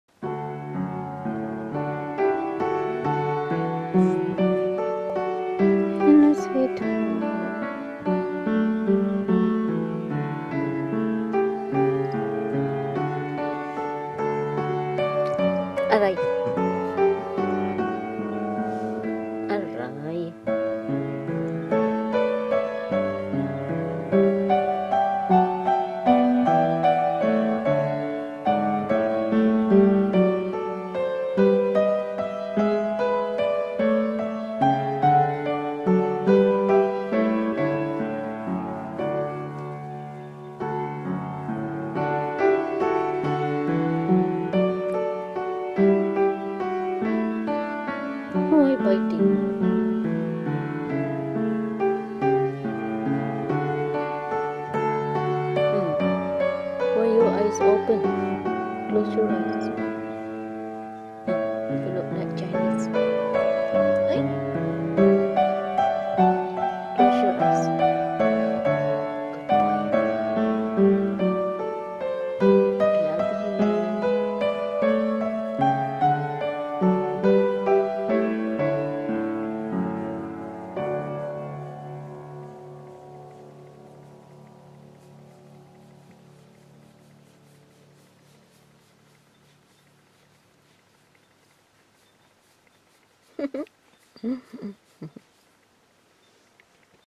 My Cat and I Got a Great Bond 😻💖 Feeling Contented and Relaxed
It's a joy that my Cat and I got a Great Bond as you can see in the video, lol. Biting is a way of showing a feeling of contentment and simply be relaxed.